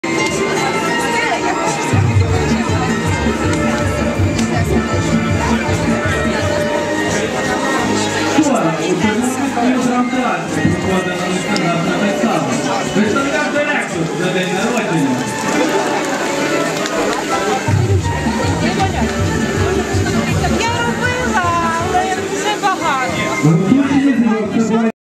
Тернопіль. Центр зайнятості
Від 30 гривень до тисячі доларів можна заробити на виробах із шерсті, які можна зробити власноруч. Безкоштовні майстер-класи по їхньому виготовленні можна було відвідати на заході, який присвятили 20-річчю служби зайнятості.